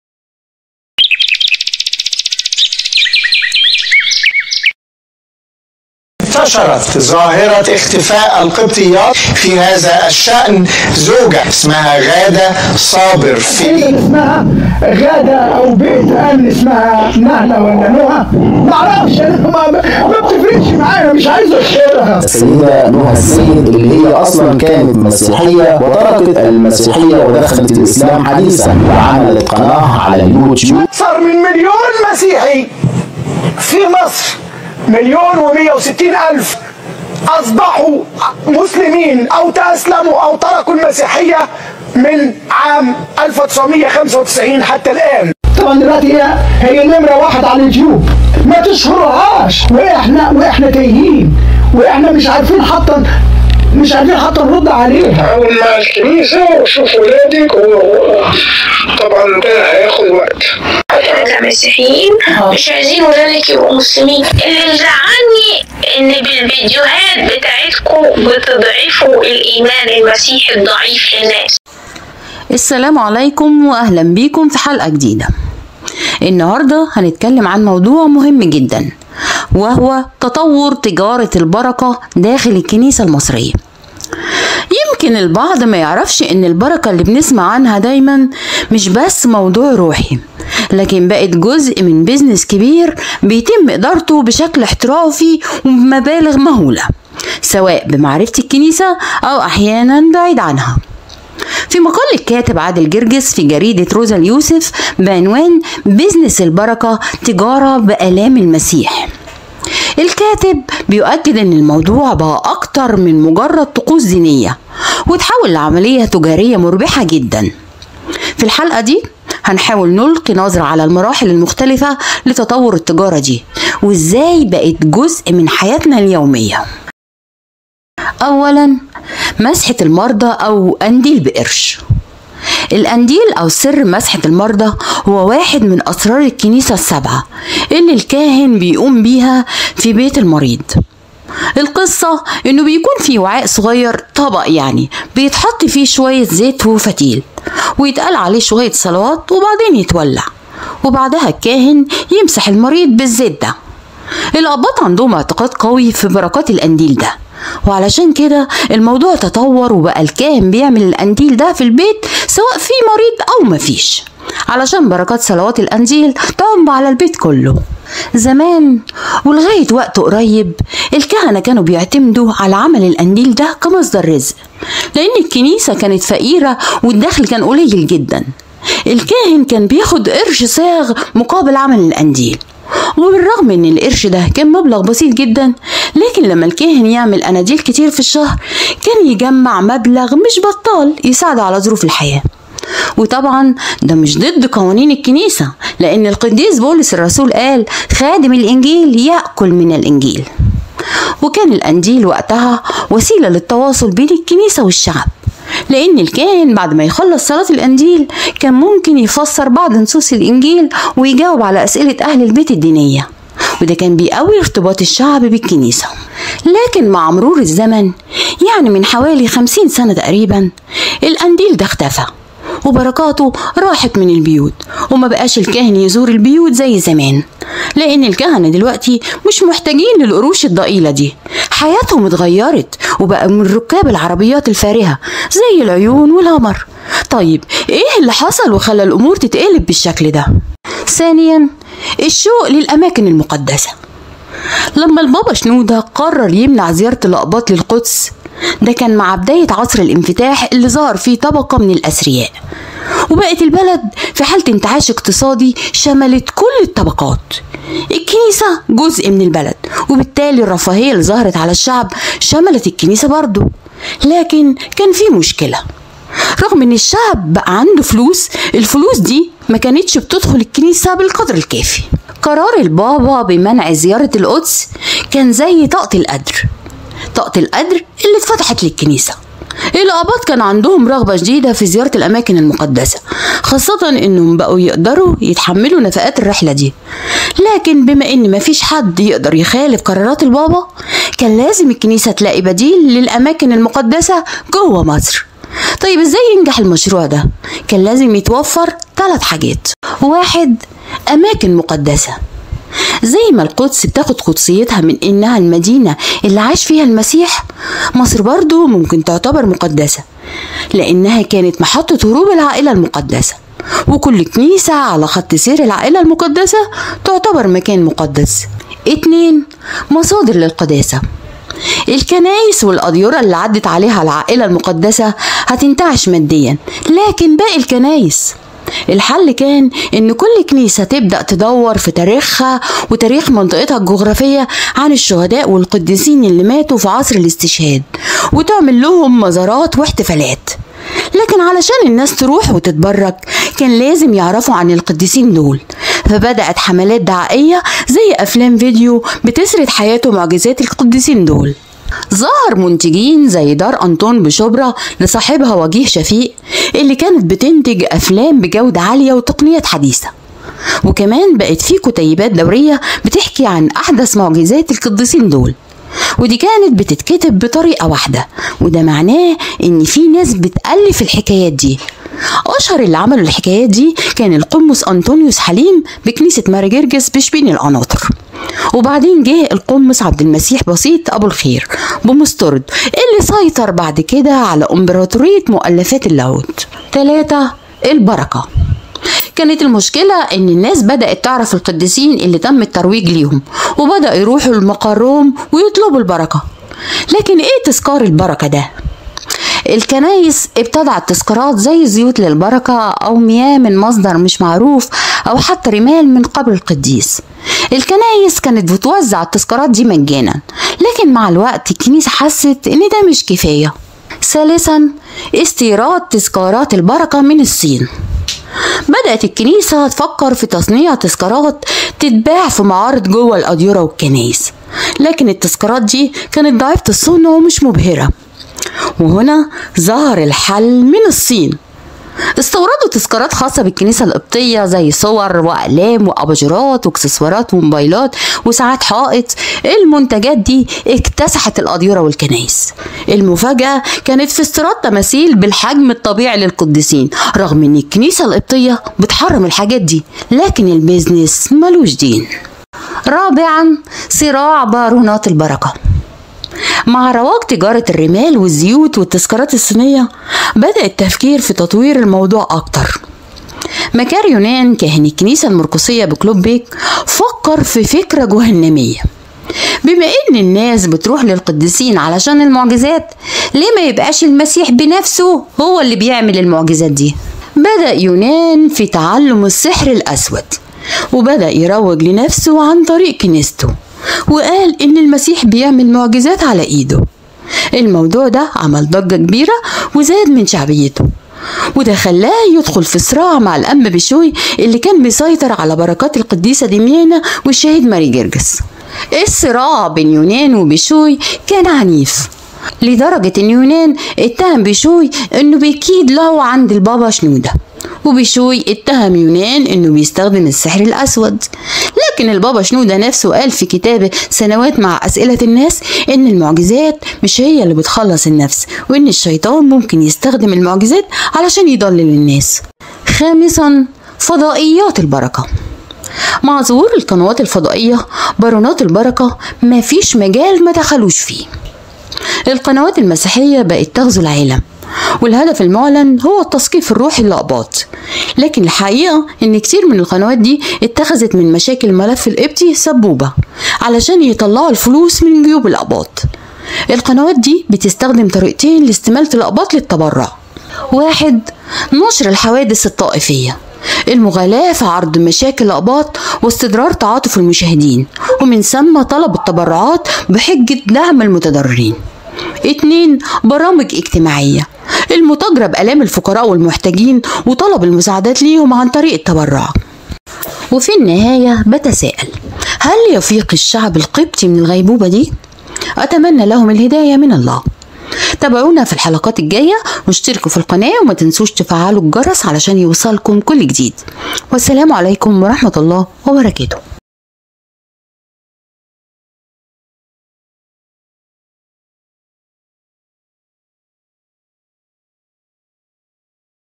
الأسرار الخفية وراء تجارة البركة في الكنيسة القبطية
في هذا الفيديو، نستعرض معكم خبايا تجارة البركة داخل الكنيسة القبطية، وكيف تحولت بعض الممارسات الدينية إلى تجارة تدر الملايين على بعض الشخصيات الكنسية. سنتحدث عن مراحل تطور هذا الظاهرة وتأثيرها على إيمان الناس وثقتهم في الكنيسة. تابعوا معنا لتعرفوا المزيد عن هذه القضية المثيرة للجدل.
رابط الباي بال الخاص بالقناة